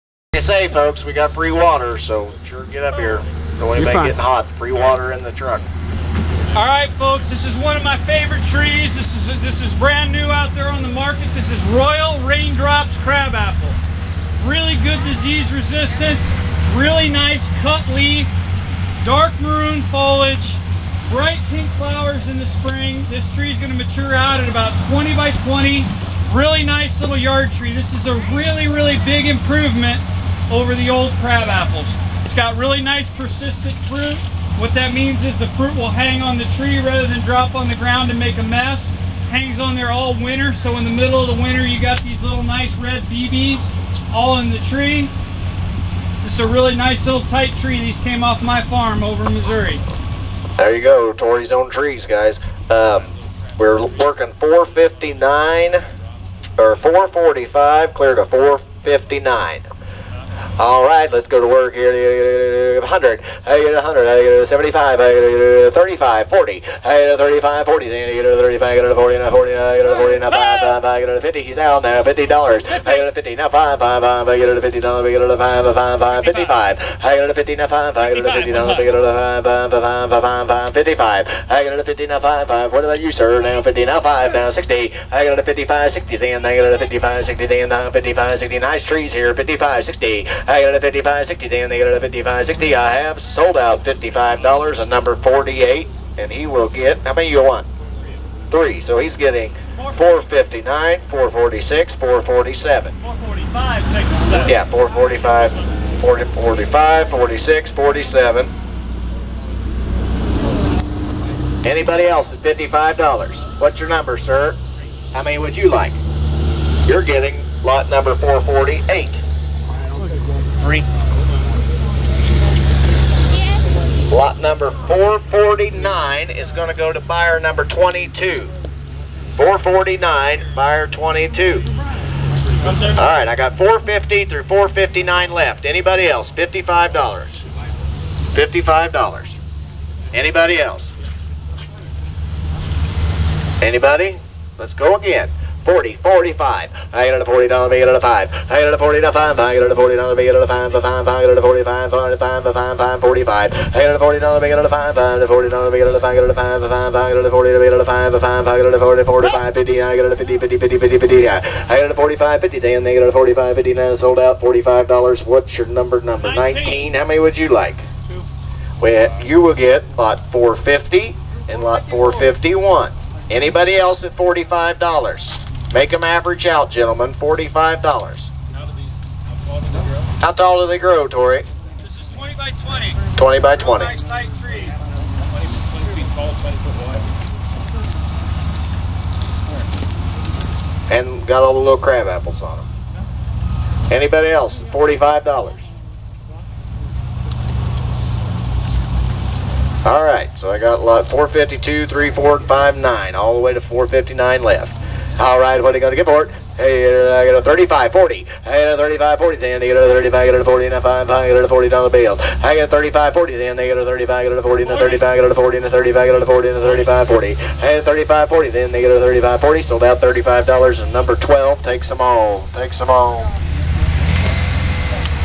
9/18 Royal Raindrops Crabapple at The Great Big Tree Auction for KC
Here you will see the auction procedure for several lots of Royal Raindrops Crabapples.  This is an example of how the auction goes for each species of tree, shrub, and perennial.  As long as you register on site and have a bidder's number, you can purchase plants at the auction very easily!